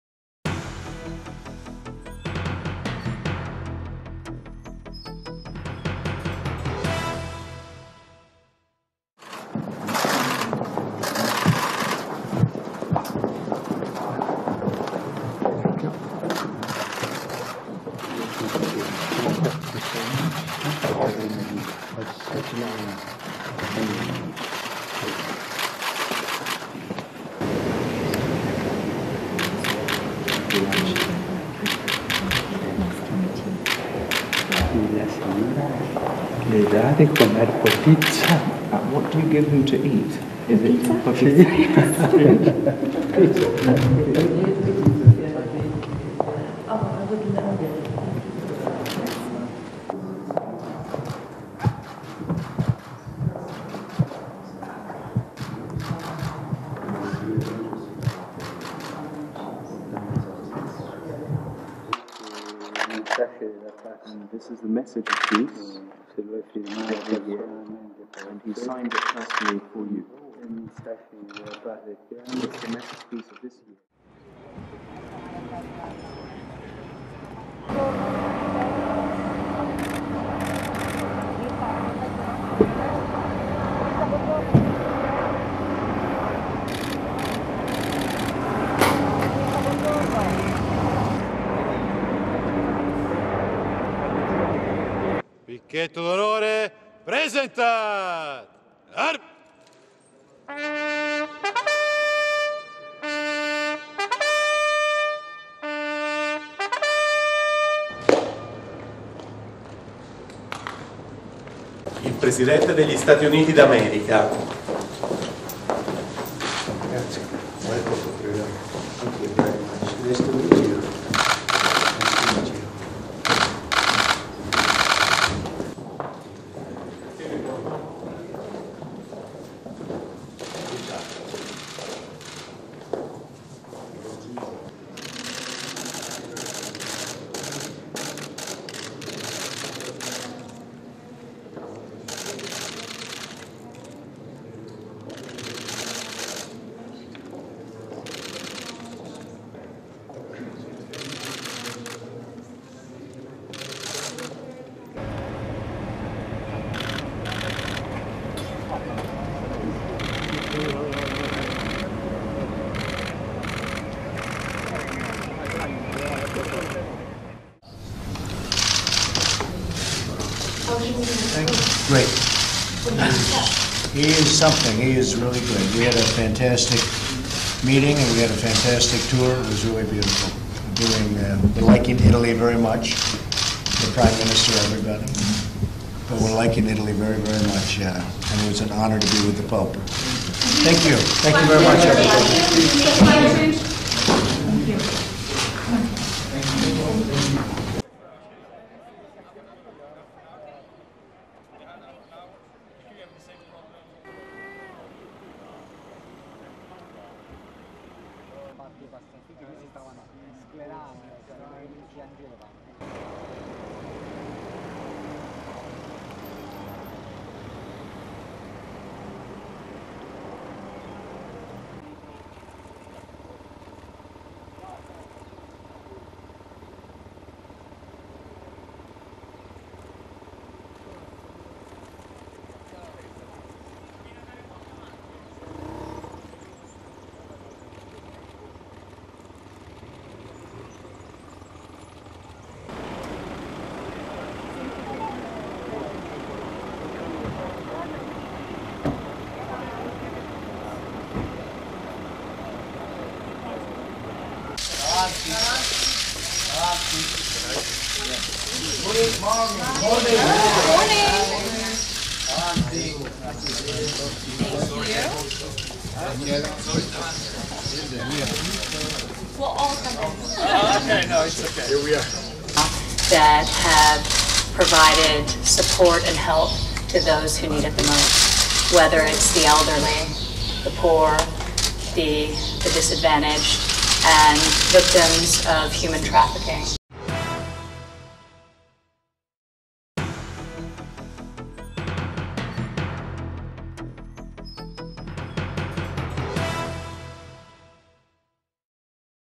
TG MONTECITORIO - LA GIORNATA POLITICA
(Agenzia Vista) Roma, 24 maggio 2017
La giornata del presidente USA Trump a Roma